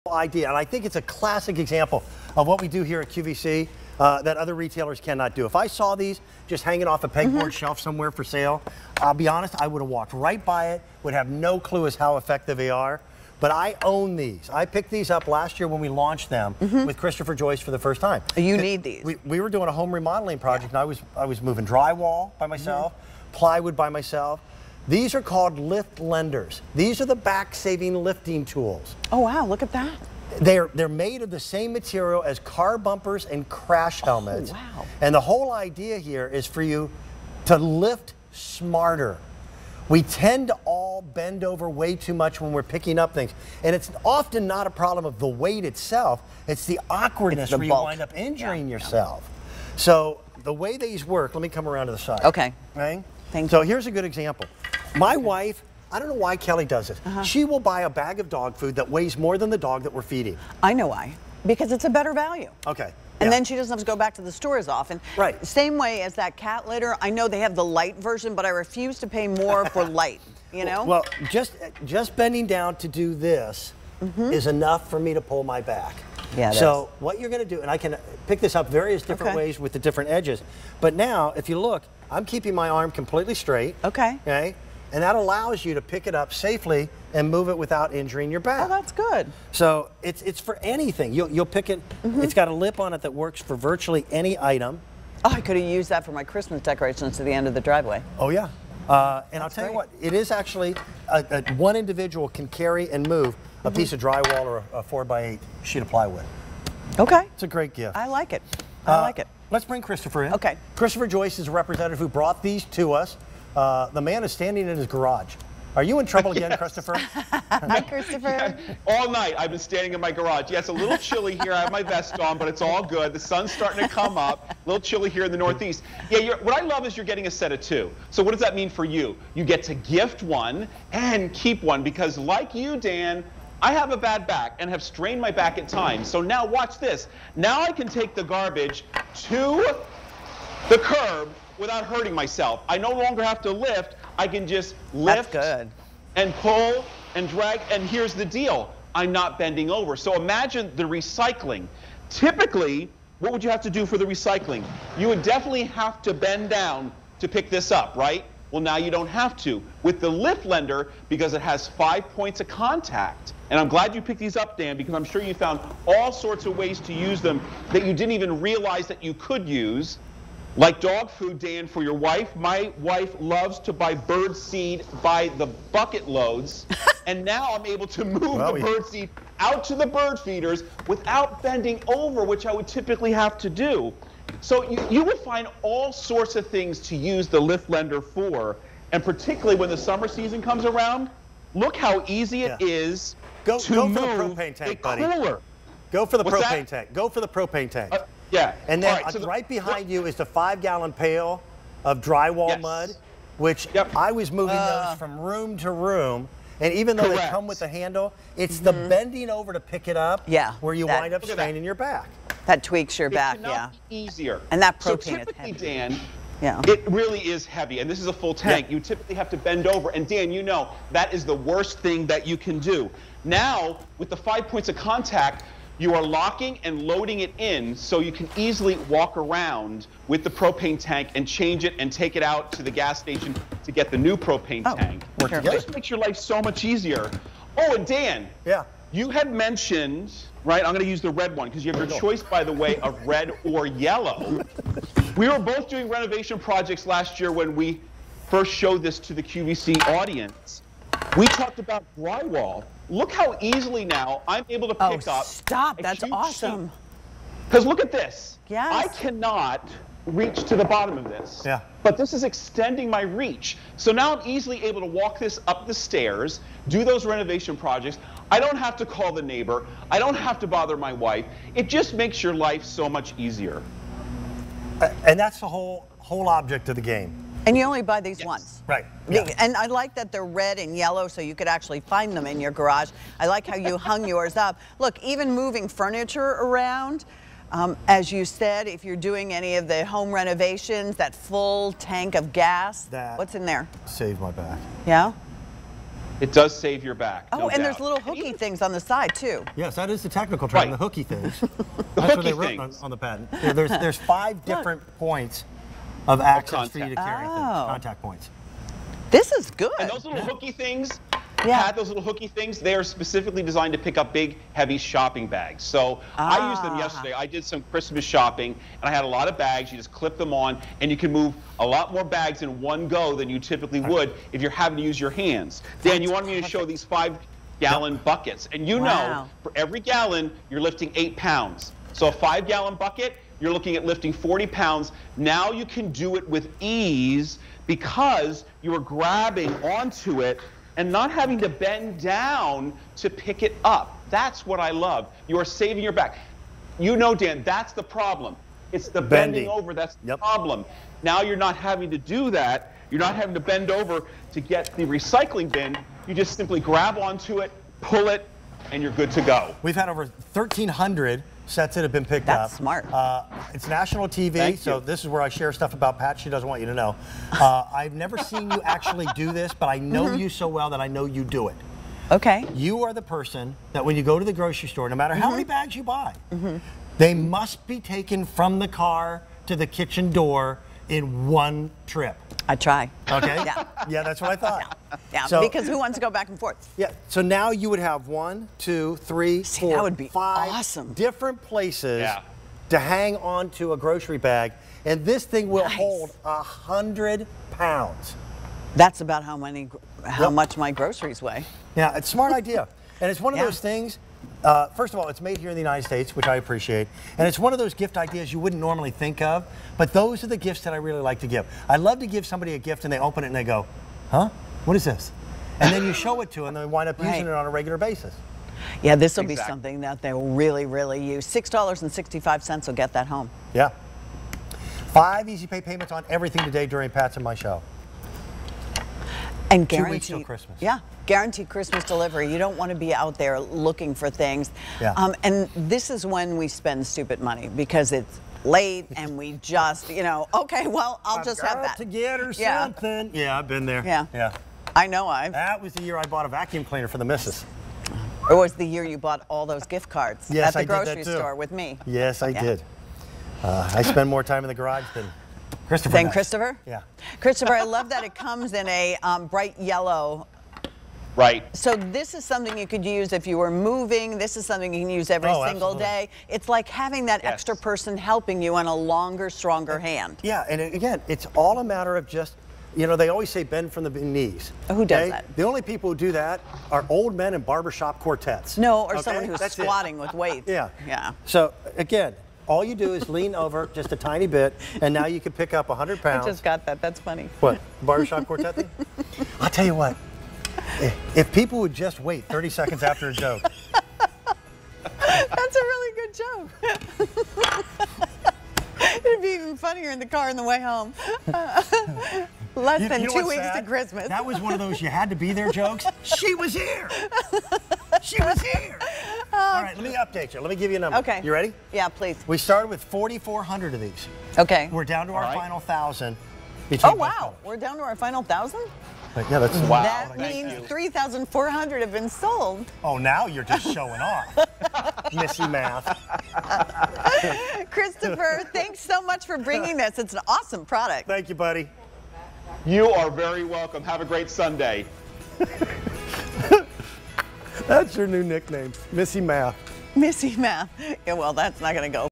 Lift Lender Set of 2 Back Saving Lifting and Carrying Tool on QVC
Lift Lender Set of 2 Back Saving Lifting and Carrying Tool

When it's time to lift, who's got your back?  Lift Lender comes to your rescue with a set of two back-saving multipurpose lift and carry tools. Acting like an extension of your arm, each tool provides you with the leverage you need to lift, reach, and carry, helping to minimize bending, stooping, and strain on your back. From Lift Lender.